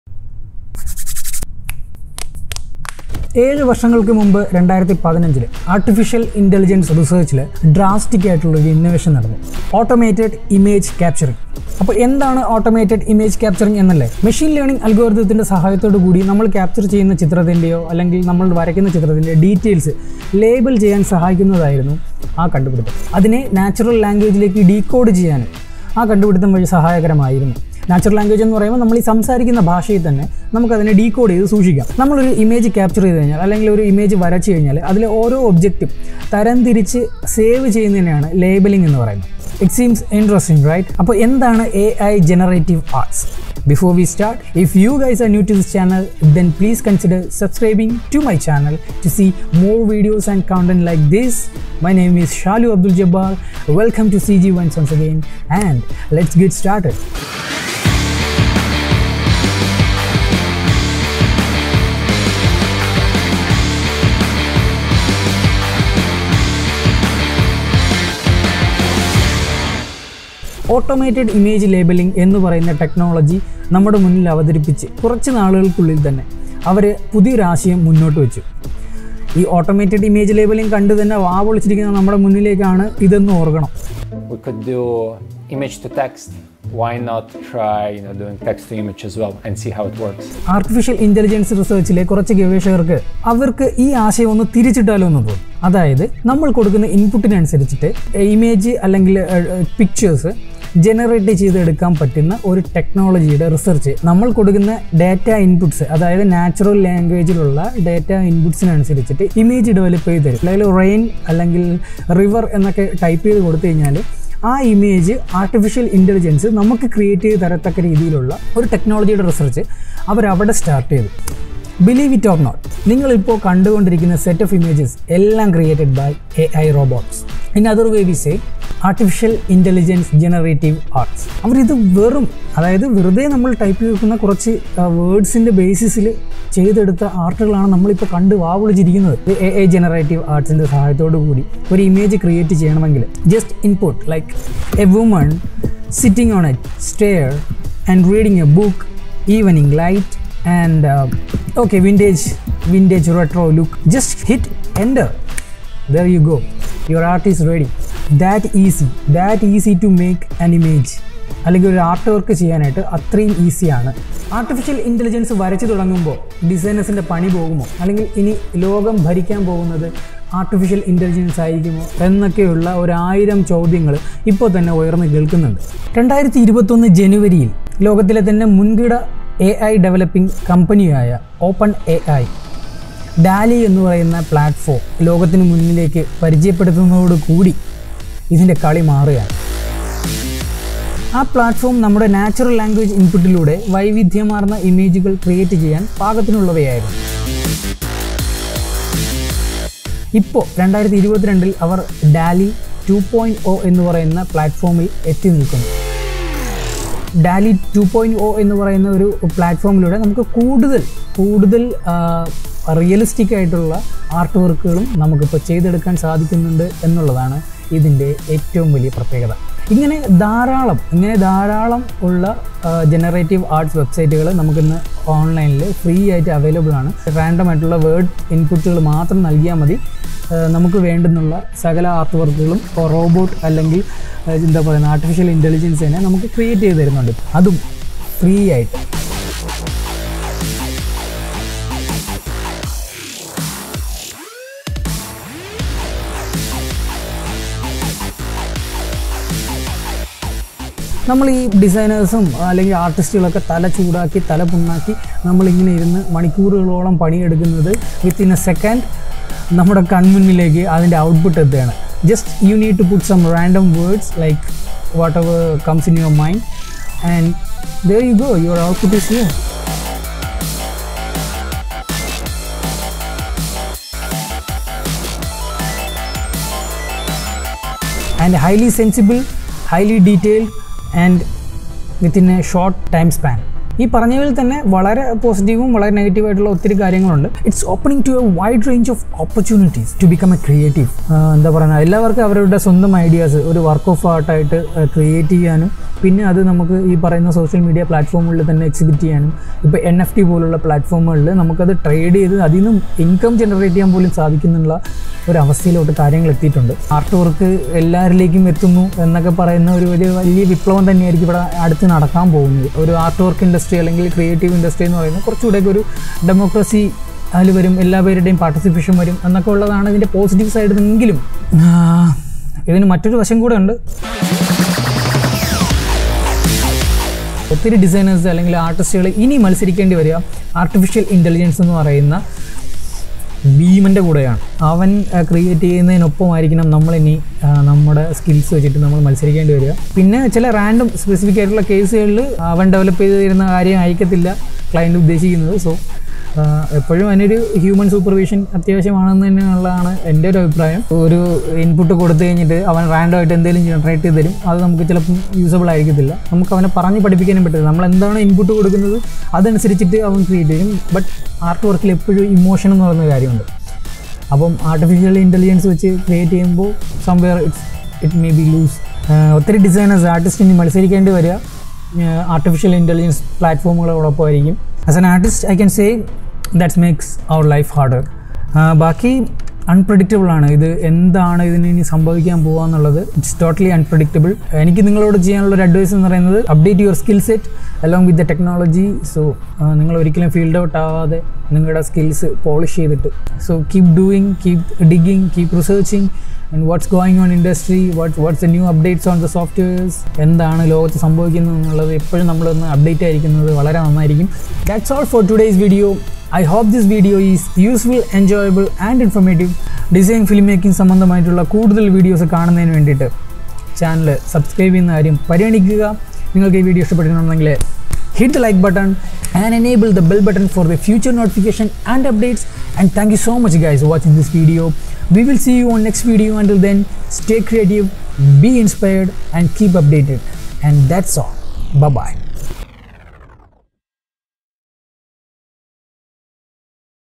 AI GENERATIVE ARTS | EXPLAINED IN MALAYALAM | CGVeins
AI Generative and future of designers /Artists. Explained in Malayalam